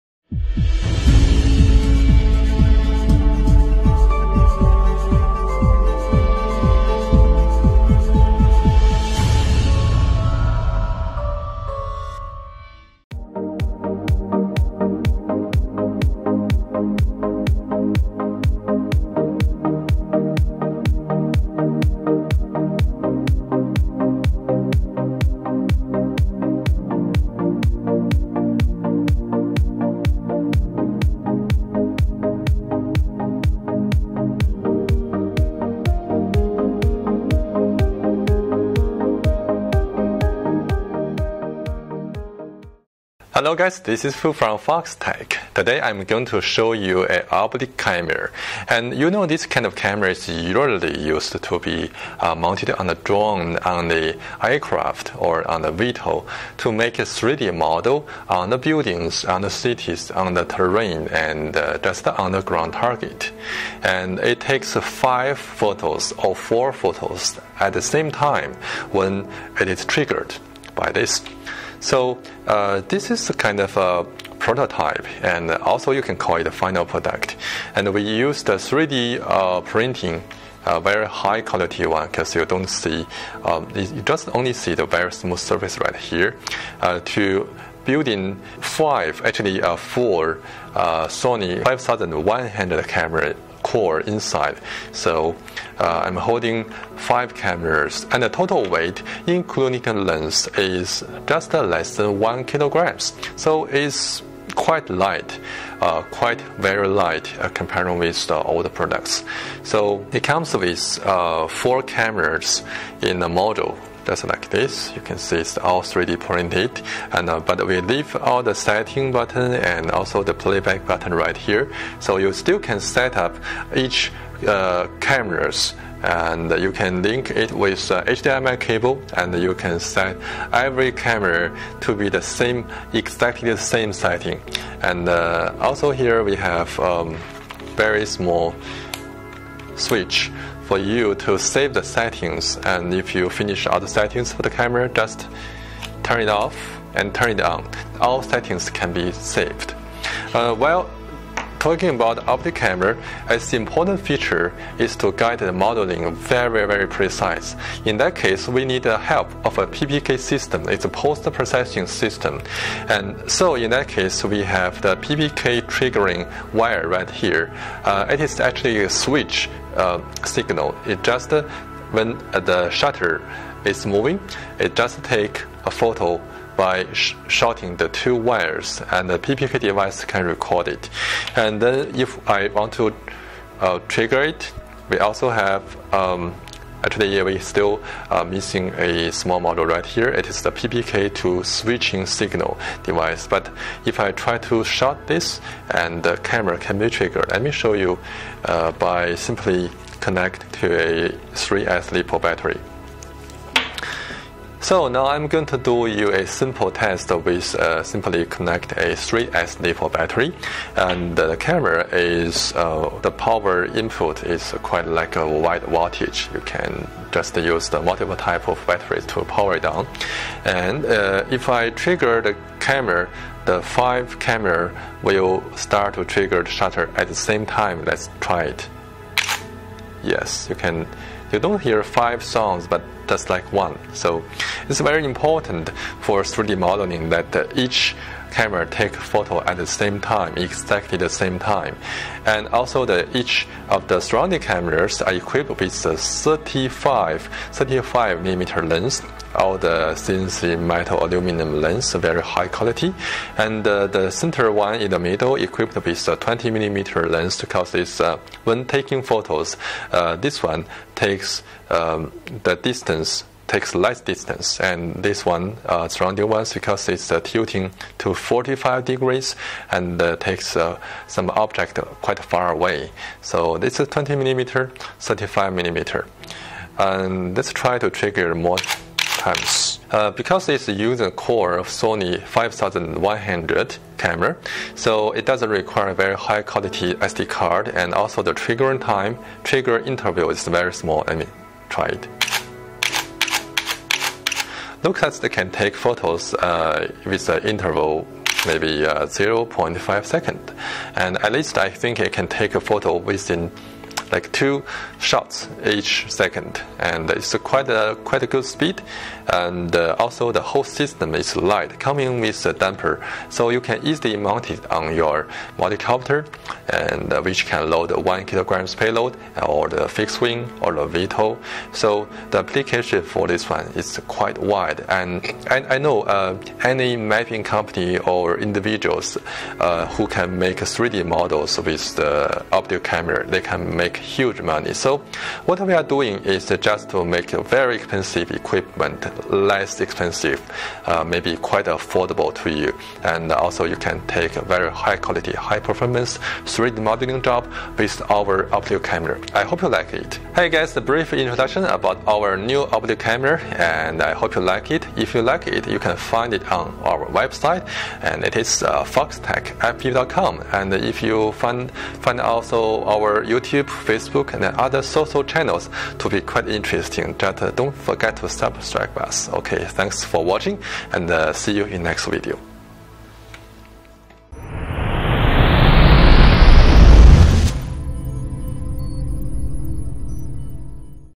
Foxtech 3DM-MINI Oblique Camera
Foxtech 3DM-MINI Oblique Camera is used to provide high resolution aerial photos to create 3D model on the city and buildings etc. It is a cost-effective Oblique multi-camera system, also a efficient equipment to maximum your productivity. The Oblique Camera integrates with 5 Sony 5100 Camera cores, it is very light, and the total weight including lens is only about 791g(nadir lens at 25mm, and oblique lens at 35mm). And the lens is interchangeable.

The surrounding cameras is equipped with 35mm lens to take subjects far away, and the center camera is equipped with 25mm lens to take less distance. With the light weight the oblique camera can be easily integrated into UAVS and aircrafts to do mapping and survey. The Oblique Camera connected with PPK system can get a very precise 3D model. And the accurate photogrammetric model can be generated by software like Pix4D, Smart3D or Photomesh.

Foxtech 3DM-MINI Oblique multi-camera system supports High level and Low level Shutter Trigger, also support Hot Shoe signal. Foxtech 3DM-MINI Oblique Camera is a good choice for companies wishing to offer high-quality photogrammetry and map-making to clients.

Specifications:

Sensor: APS-C Exmor CMOS Sensor(23.5x15.6mm)
Number of Pixels: 24MP
Total Pixels: 120MP
Image Size: 6000x4000
Lens compatibility: E-mount lens
Hot Shoe Signal: Supported
Shutter Trigger: High level/Low level
Lens: Interchangeable
Data Reading: SD card/USB2.0
Storage: SD Card
Parameter Setting: Button/USB
Power: 9-28V
Size: 14.5x14.5x7cm(no lens)
Weight:791g(5 lens); 635g(4 lens)